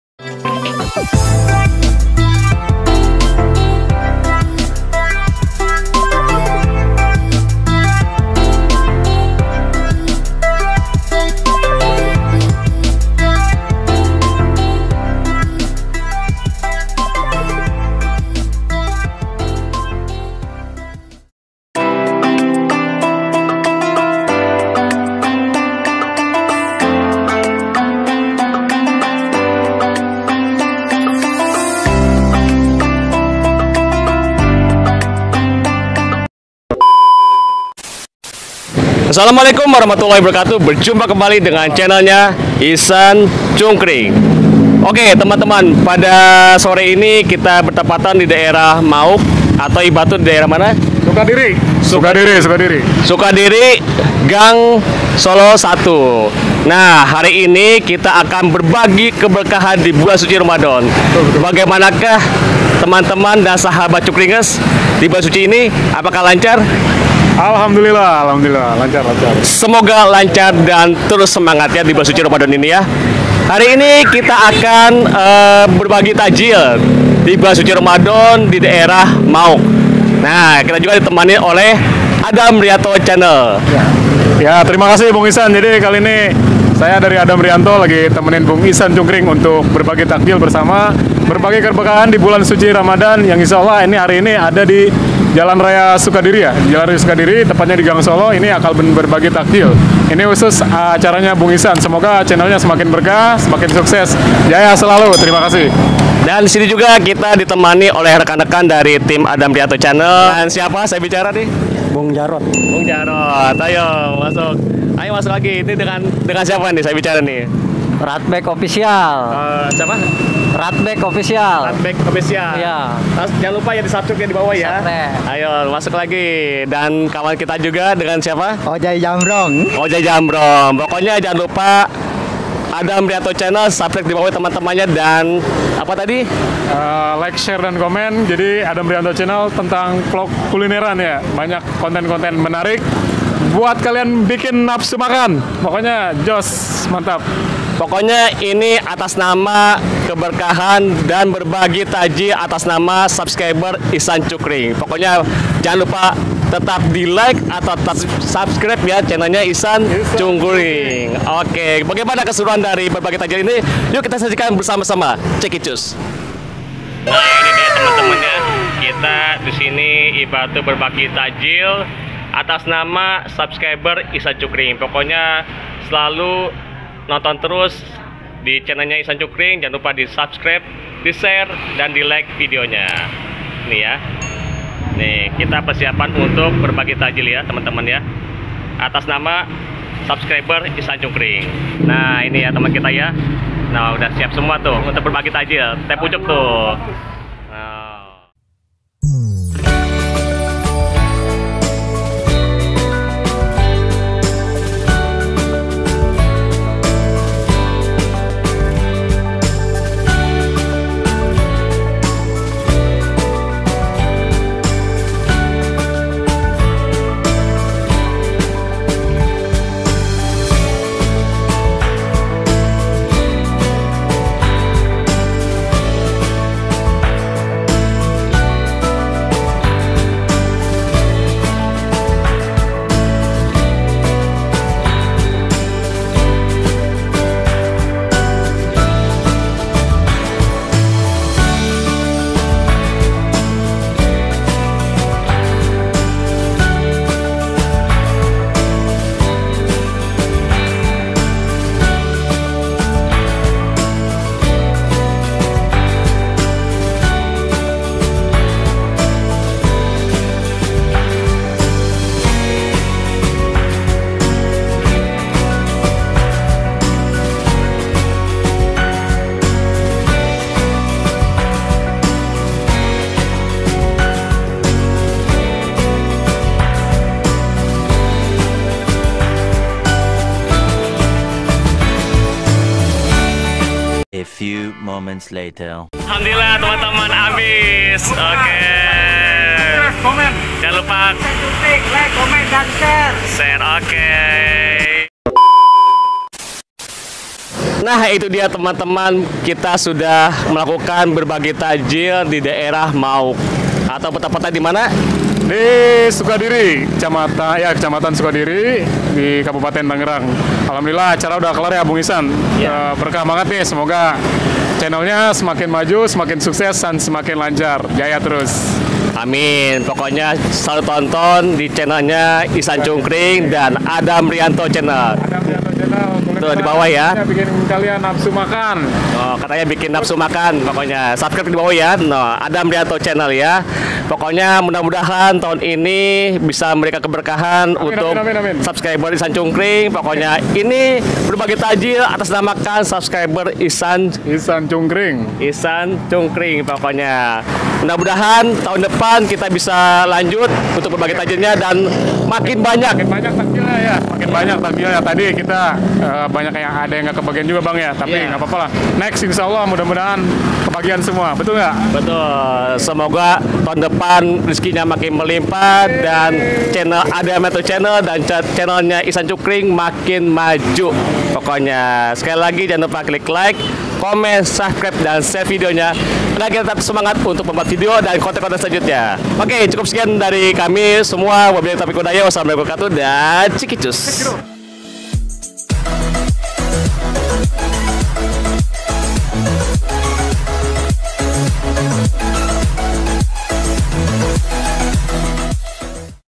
BERBAGI KEBERKAHAN TAKJIL DI BULAN SUCI RAMDHAN 1443 H DARI SUBSCRIBERS ISAN CUNGKRING
hallo sahabat "THE CHUNGKRINGERS".
bagaimana kabar kalian semua. ?? semoga dalam keadaan sehat walafiat. amien
episode kali ini kita akan BERBAGI KEBERKAHAN TAKJIL DI BULAN SUCI RAMADHAN 1443 H DARI SUBSCRIBERS ISAN CUNGKRING.
Acara tersebut ucapan rasa syukur kepada subscribers isan cungkring yang selalu support channelnya isan cungkring. semoga tahun depan berbagi takjil nya makin banyak dan channelnya juga semakin maju. amien